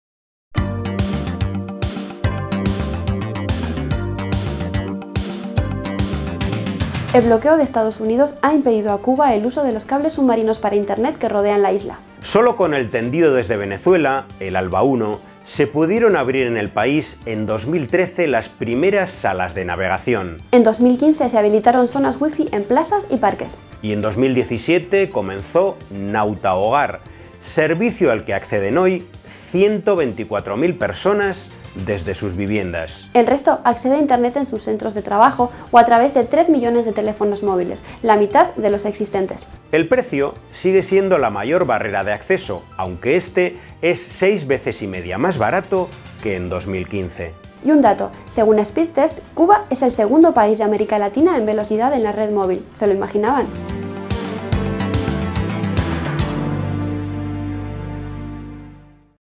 Cuba a velocidad… de Internet
El bloqueo de EEUU ha impedido a Cuba el uso de los cables submarinos para Internet que rodean la Isla. Espacio "Contratuit", en Cubainformación TV. Fuente